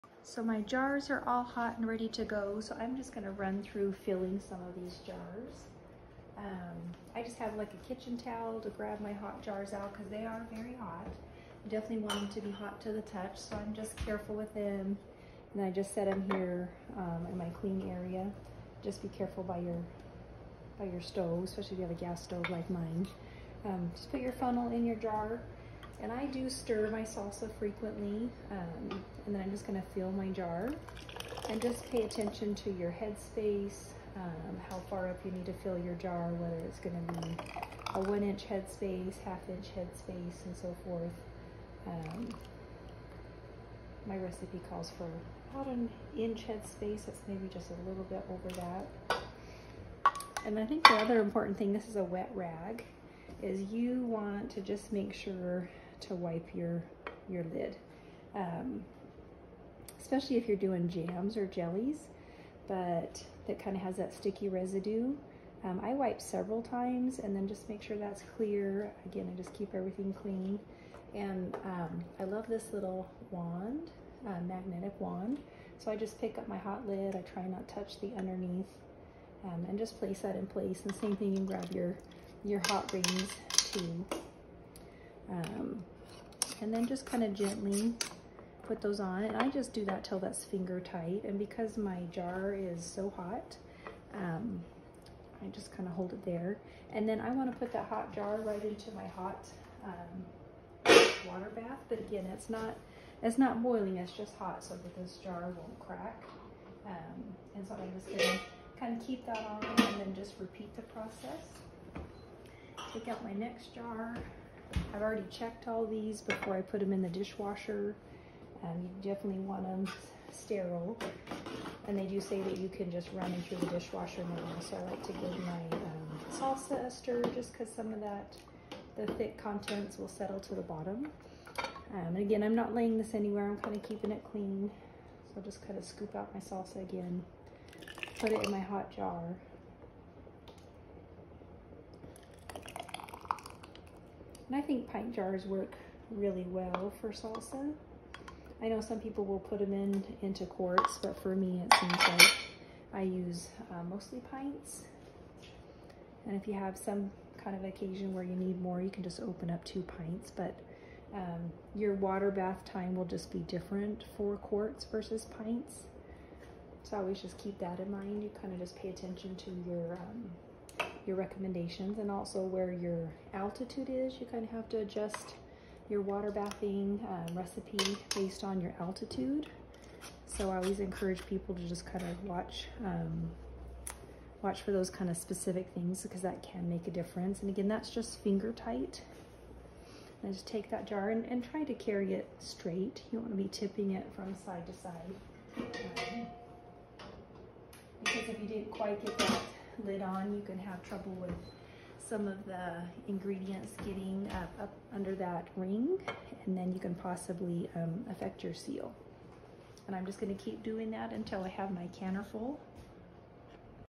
Canning steps
Canning steps for hot water bathing canned salsa.